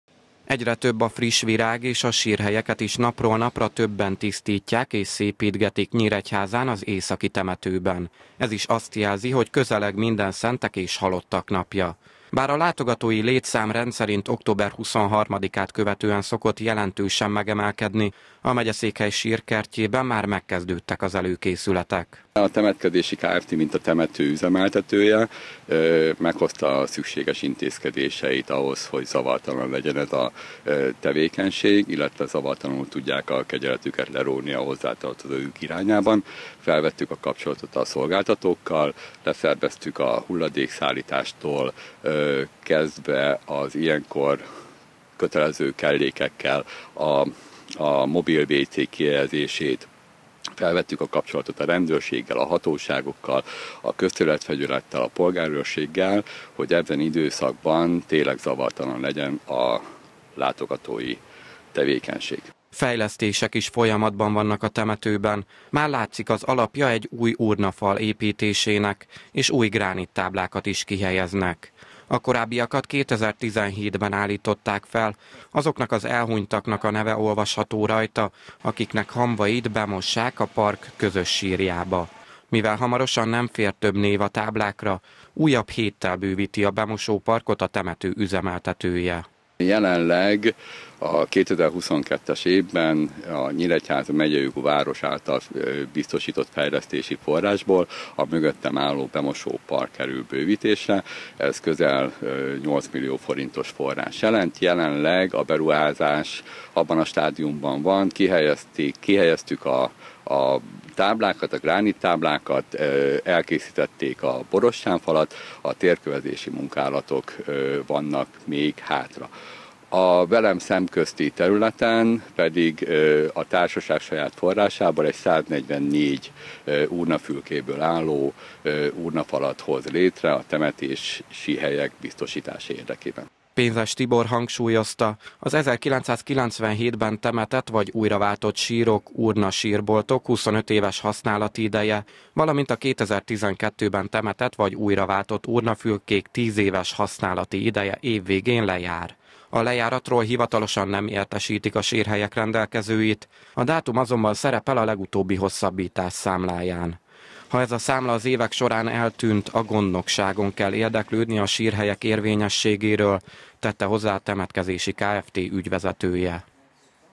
Készülnek Mindenszentekre
Alig több mint egy hét van hátra Mindenszentekig és Halottak napjáig. A nyíregyházi Északi temetőben már megkezdték az előkészítő munkálatokat annak érdekében, hogy zavartalanul tudják fogadni azt a több tízezer látogatót, akik megemlékeznek elhunyt hozzátartozójukról.

A sírkertben fejlesztések is folyamatban vannak, új urnafalat építenek, a bemosó-parkot pedig hét új gránittáblával bővítik.